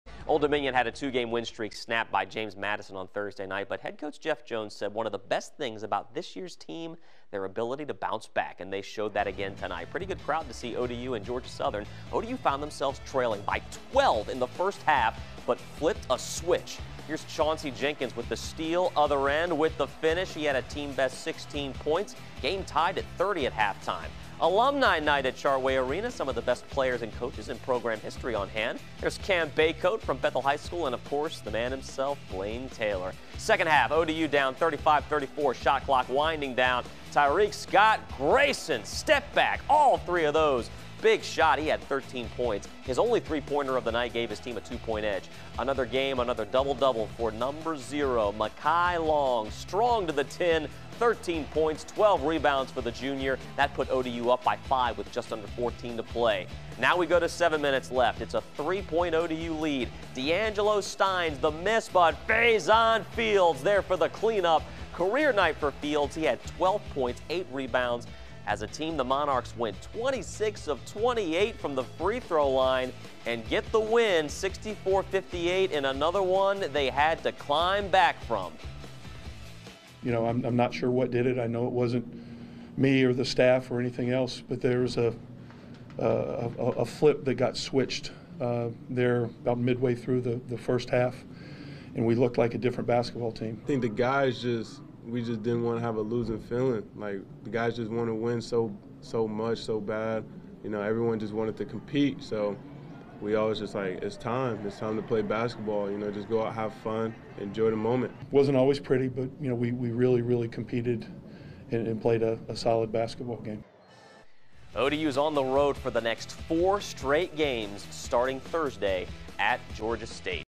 ODU rallies, earns 64-58 win over Georgia Southern
Old Dominion trailed by 12 in the first half, but rallied for a 64-58 win over visiting Georgia Southern at Chartway arena.